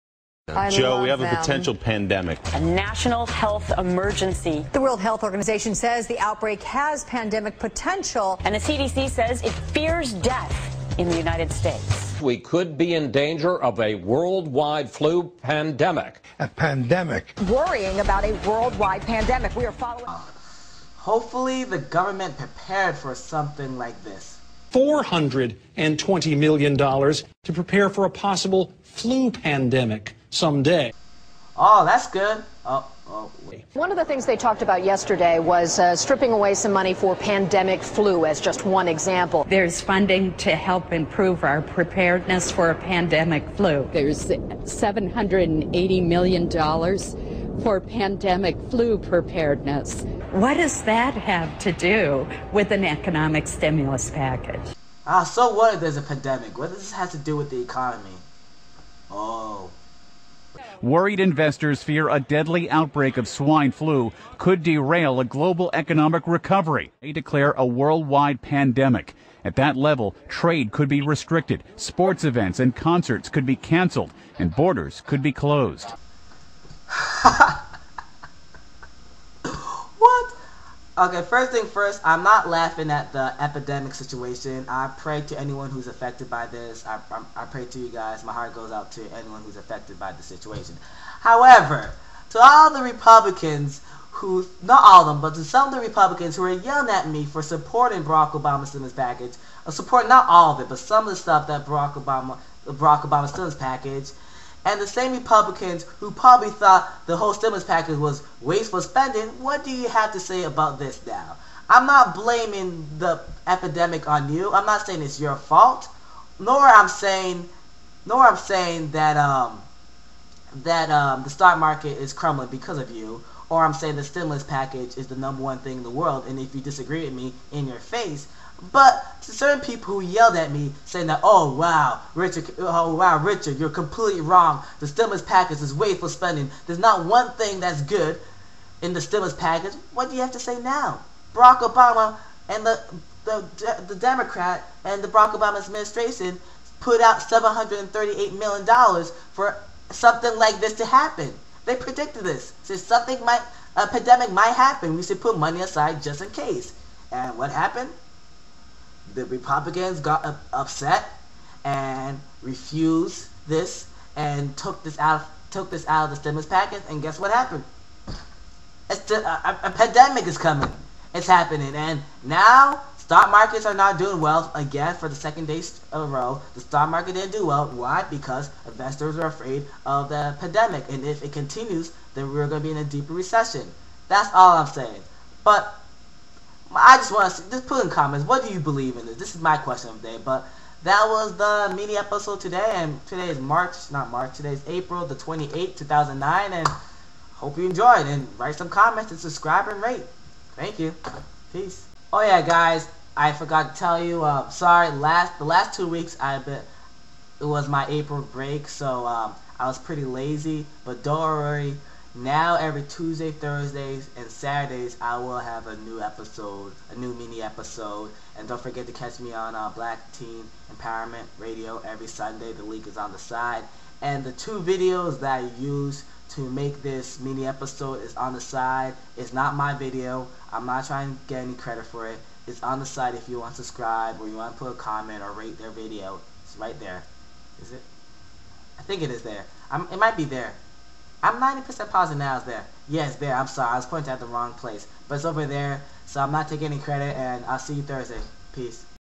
Certain people were Against putting Money to Prepare for Pandemics -- WOOW
What!!!? 738 million dollars of stimulus package was supposed to fund programs to Prepare for Pandemics like the one now but people said noo because they believe it was a waist of money. WOOOW thats a big opps